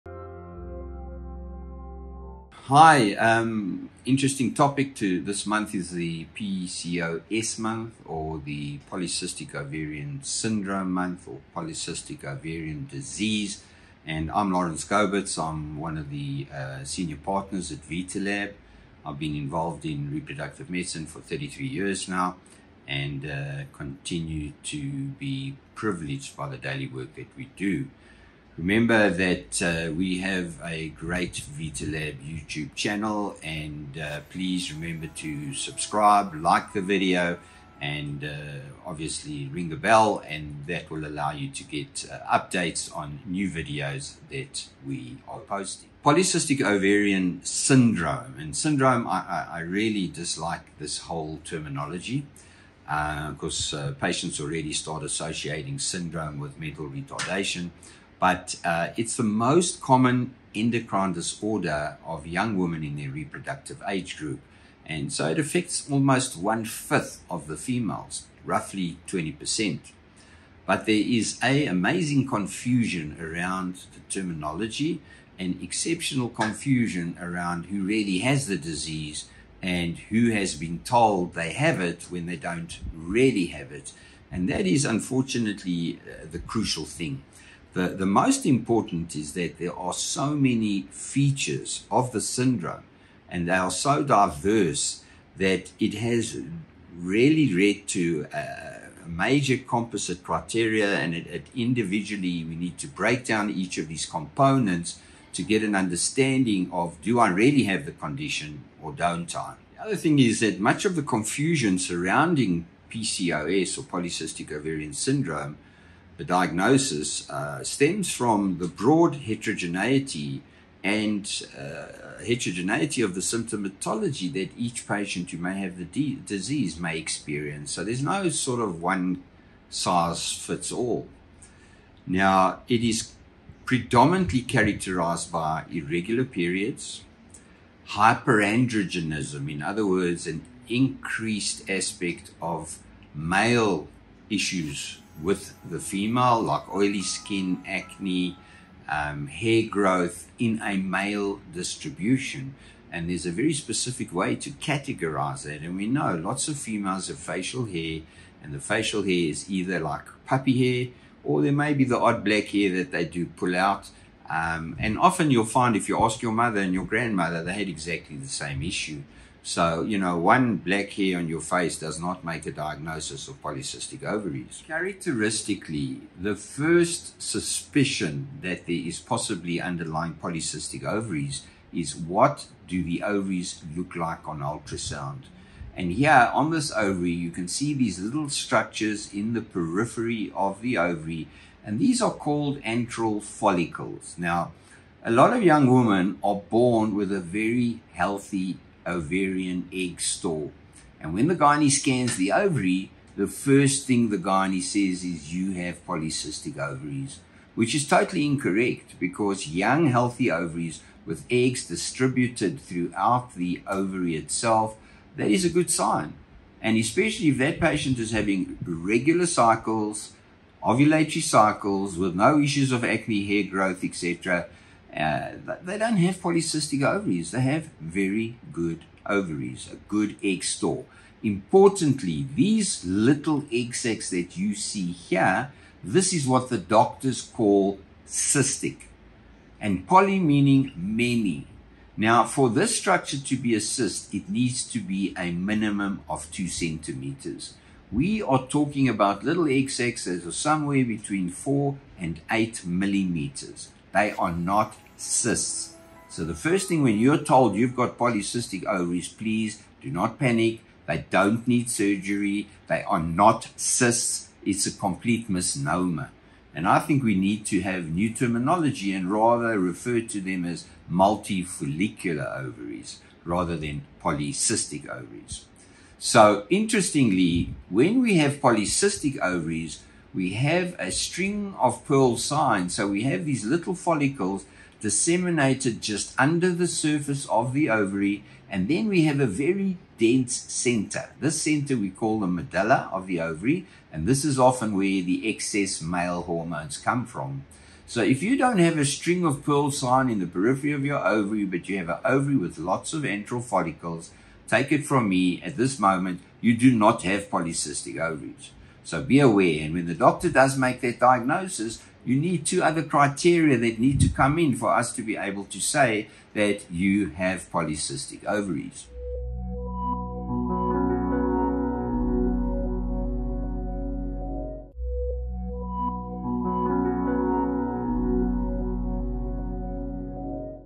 Dr Gobetz discusses PCOS
September is PCOS Awareness Month, and we're shining a spotlight on this diagnosis with Dr Gobetz, taking you through what it is, what to look out for and whats important to know.

💛 What is PCOS? 💬
Polycystic Ovary Syndrome affects millions of individuals worldwide. It's a hormonal disorder that can lead to various challenges like irregular periods, fertility issues, and more. Let's educate ourselves and spread the word! 📚